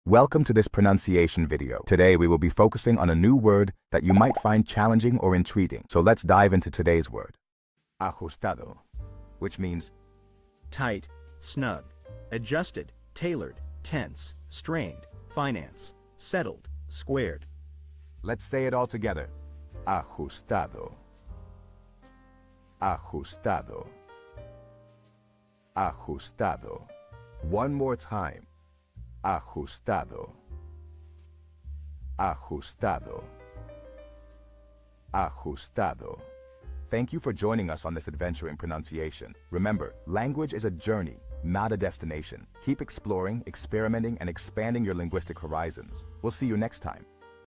How to pronounce Ajustado
Master the Pronunciation of 'Ajustado Which means adjusted' - which means : Tight, snug;adjusted, Tailored;Tense, Strained;(Finance) Settled, Squared. 📝 with @PronunciationSpanish 🎓🗣️ - [Your Guide to Spanish]

Learn to pronounce 'Ajustado' perfectly in Spanish with our easy and engaging tutorial by @PronunciationSpanish! 📖📽️ In this comprehensive video, we provide you with native speaker examples 🗣️👂 and phonetic breakdowns 🎵 to ensure you get the pronunciation just right.

How to say Tight in Spanish